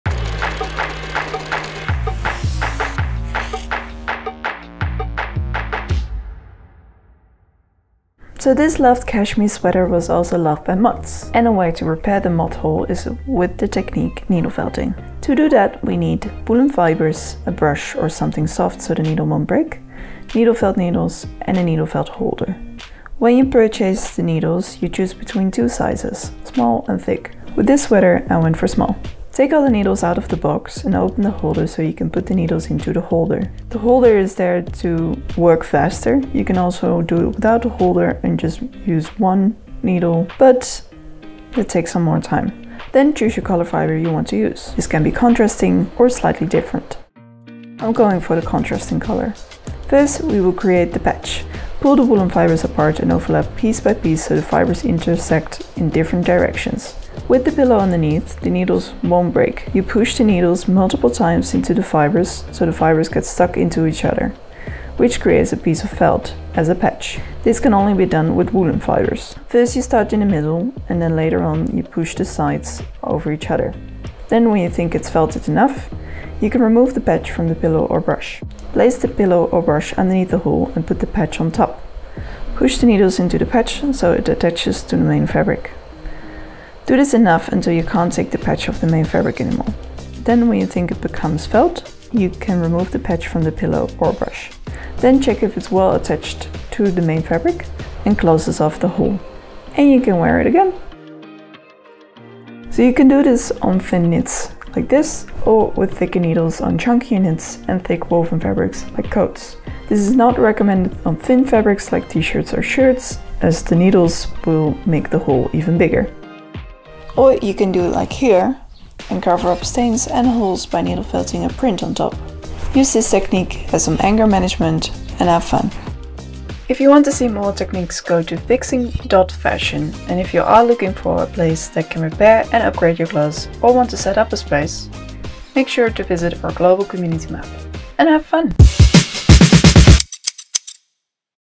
Fix hole in woolen sweater (Needle Felting technique)
See more about Fixing Fashion and its collection to repair and upgrade clothes